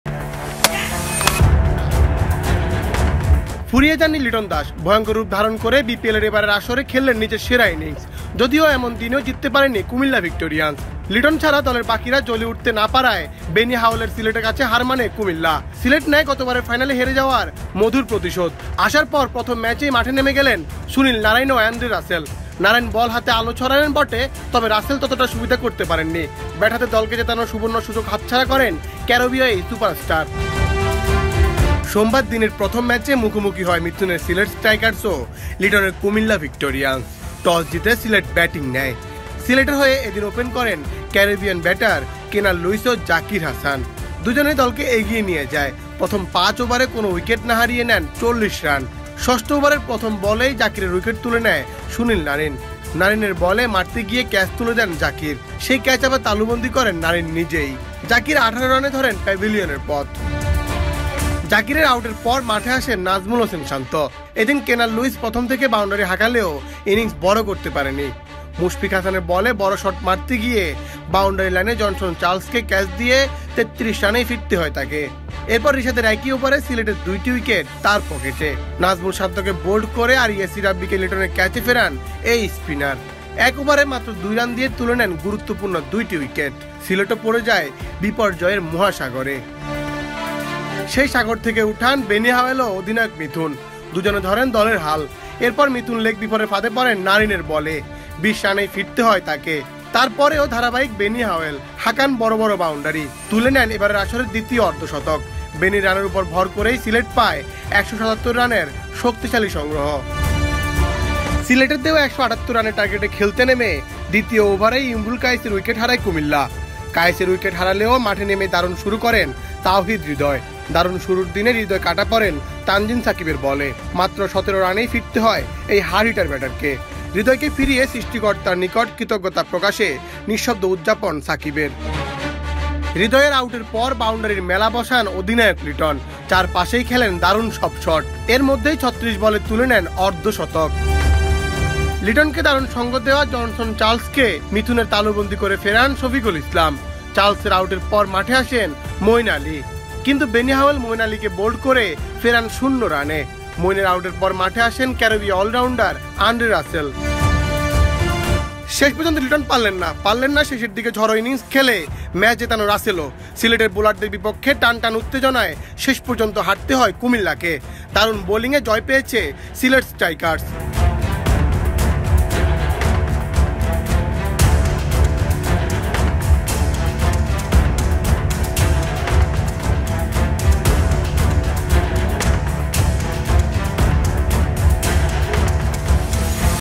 লিটনের ৮৫ রানের পরও হারল কুমিল্লা, দলকে জেতাতে পারলেন না রাসেল, তানজিম সাকিবের বাজিমাত | Litton
ফুরিয়ে যাননি লিটন দাস, মোটেও ফুরিয়ে যাননি। বরং আরও ভয়ংকর রুপ ধারণ করে বিপিএলের এবারের আসরে খেললেন নিজের সেরা ইনিংস। যদিও এমন দিনেও জিততে পারেনি কুমিল্লা ভিক্টোরিয়ান্স। লিটন ছাড়া দলের বাকিরা জ্বলে উঠতে না পারায় বেনি হাওয়েলের সিলেটের কাছে হার মানে কুমিল্লা, সিলেট নেয় গতবারের ফাইনালসহ এবারের আগের দেখায় হেরে যাওয়ার মধুর প্রতিশোধ। আসার পর প্রথম ম্যাচেই মাঠে নেমে গেলেন সুনিল নারাইন ও আন্দ্রে রাসেল। নারাইন বল হাতে আলো ছড়ালেন বটে, তবে রাসেল ততটা সুবিধা করতে পারেননি। ব্যাট হাতে দলকে জেতানোর সুবর্ণ সুযোগ হাতছাড়া করেন ক্যারিবীয় এই সুপারস্টার।